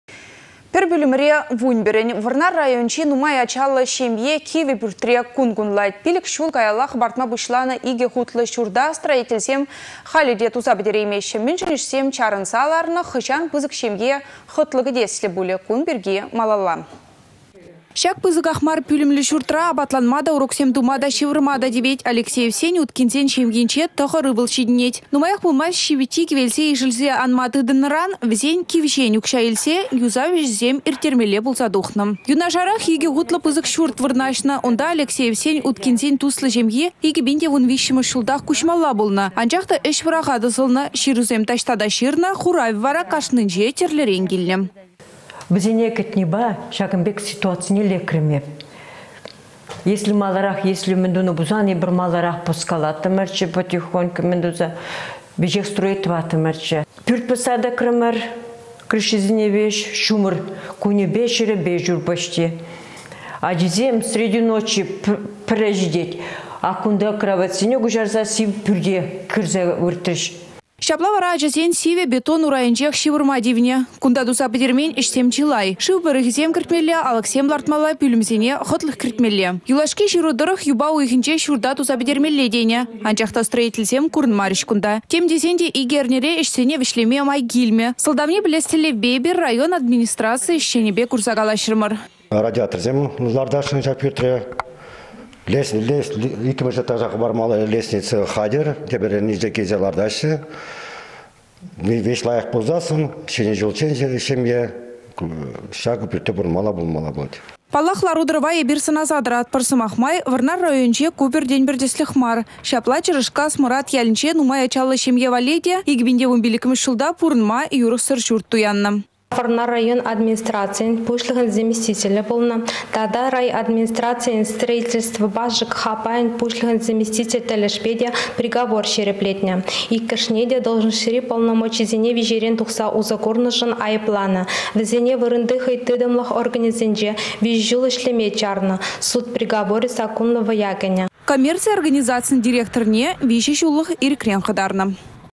Вунпĕрĕн пĕр пÿлĕмре
Пĕр пÿлĕмре вунпĕрĕн. Вăрнар районĕнчи нумай ачаллă çемье кивĕ пÿртре кун-кунлать. 5 çул каялла хăпартма пуçланă 2 хутлă çурта строительсем халĕ те туса пĕтереймеççĕ. Мĕншĕн ĕçсем чарăнса ларнă? Хăçан пысăк çемье хăтлă кĕтеслĕ пулĕ? Кун пирки малалла.